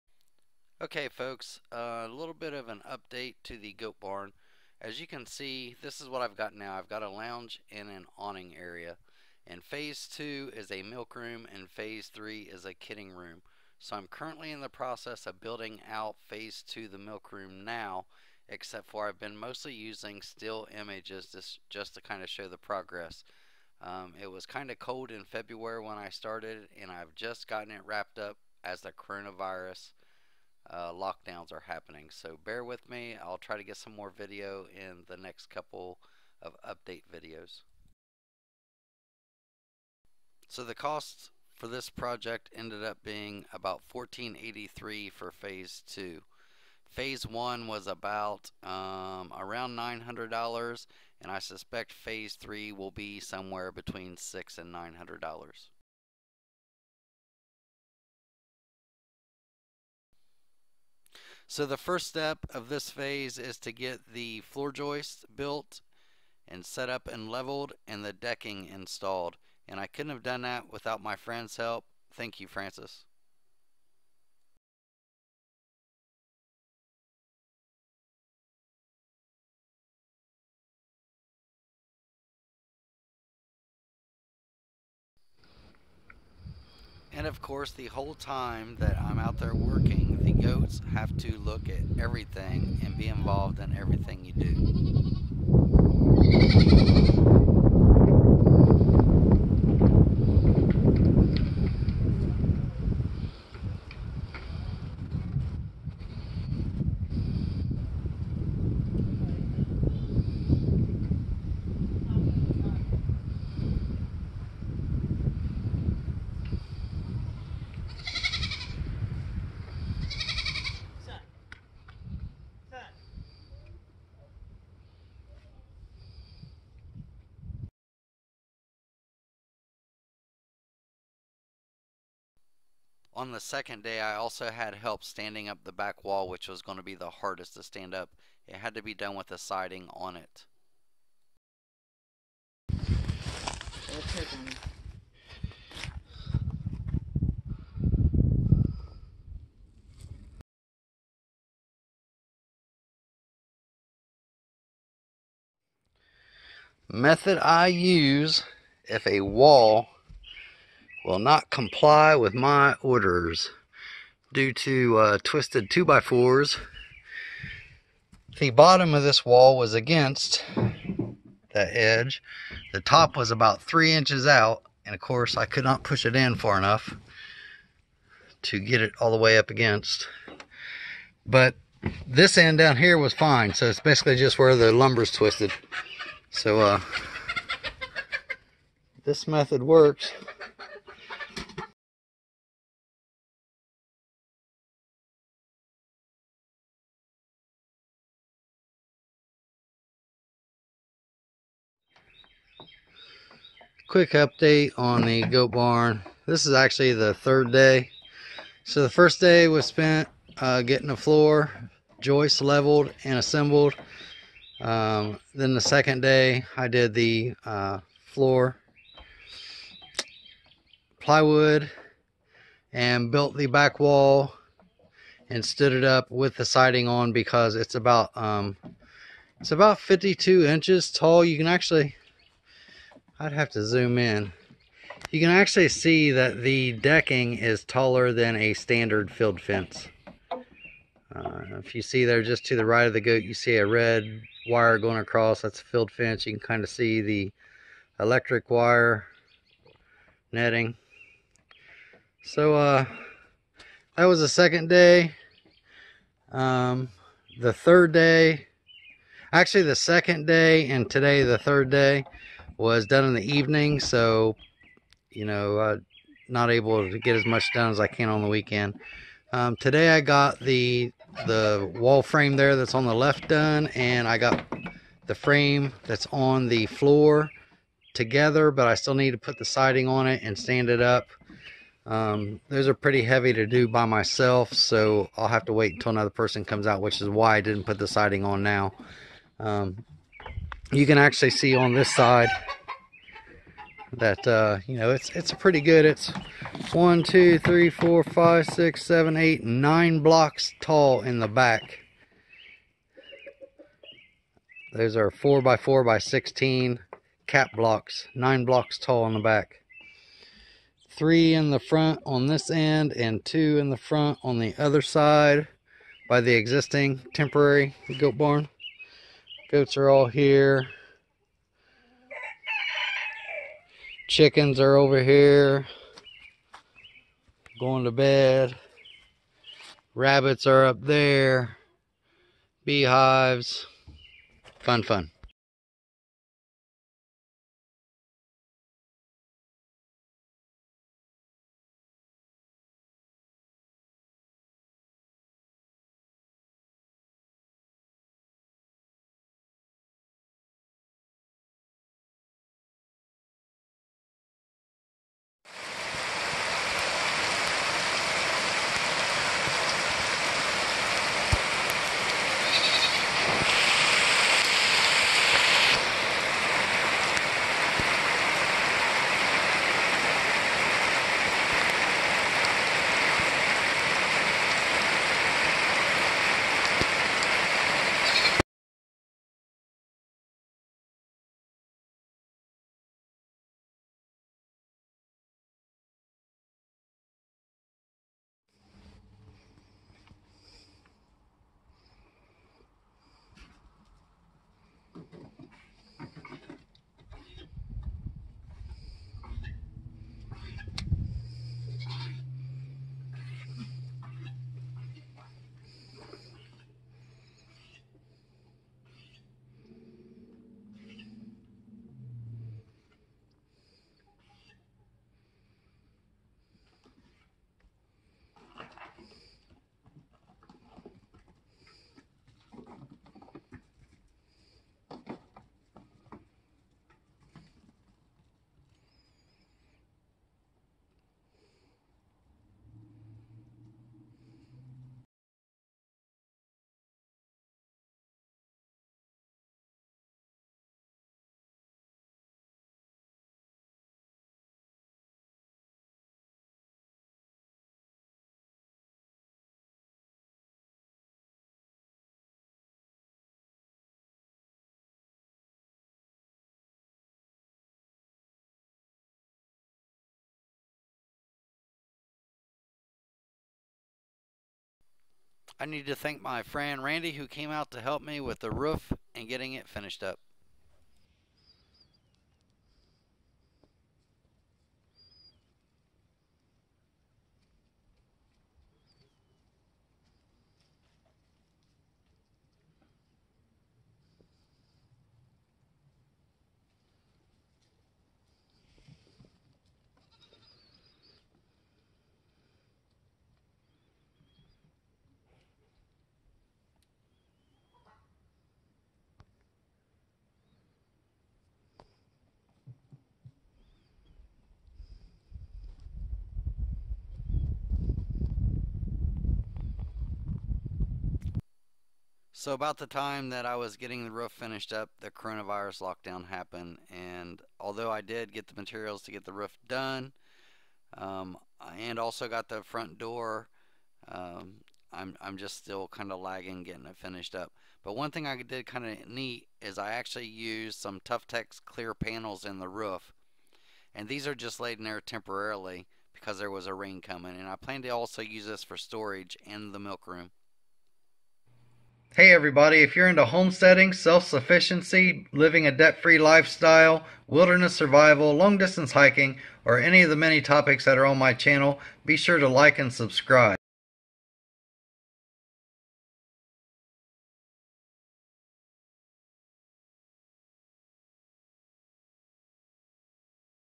homesteading goat barn phase 2 milk & storage room build
I literally started to build this at the end of february, right before all the coronavirus stuff was about to happen. Although I had most of the materials already purchased, there are a few things I need to finish up cosmetically but as time drags on today is April 17 and I haven't left my house since the first week of march. I figured I could go ahead and post what I have so far ... because all that is really lacking for phase 2 are pictures of installing the door and finish trim. Hope you enjoy ... because I was on a timeline to get this done and with limited evening hours to work in, I opted to not record as much video and do still however, you will see I did get a decent amount of video too.